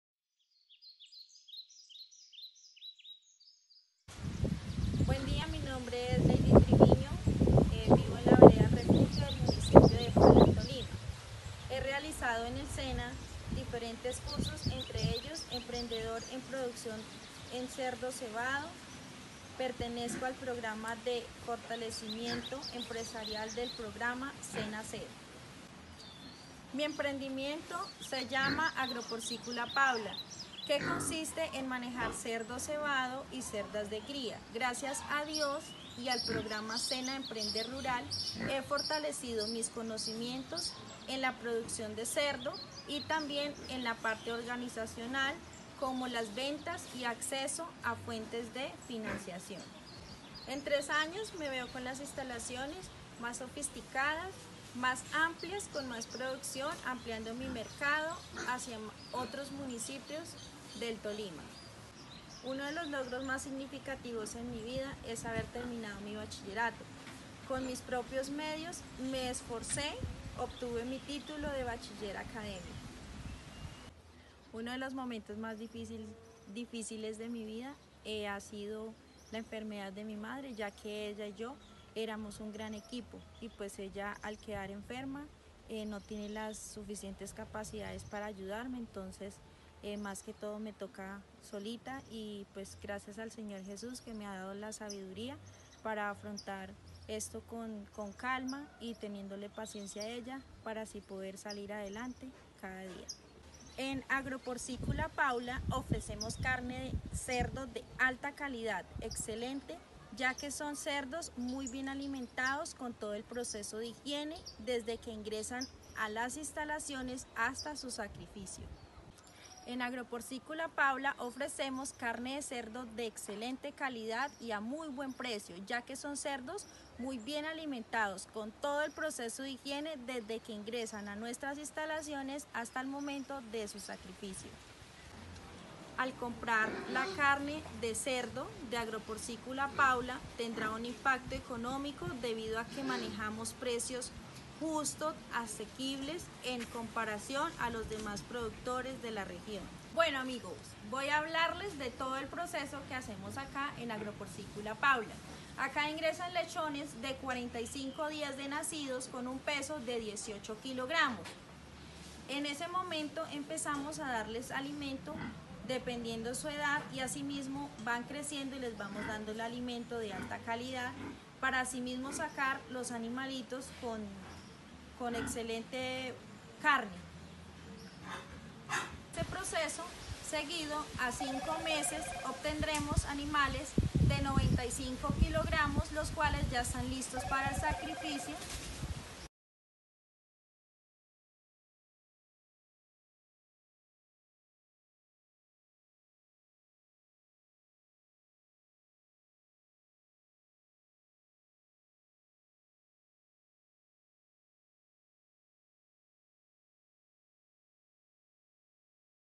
122420_PORCÍCOLA PAULA_2022
UP Porcícola Paula dedicada a la cría de cerdos. Falan - Tolima.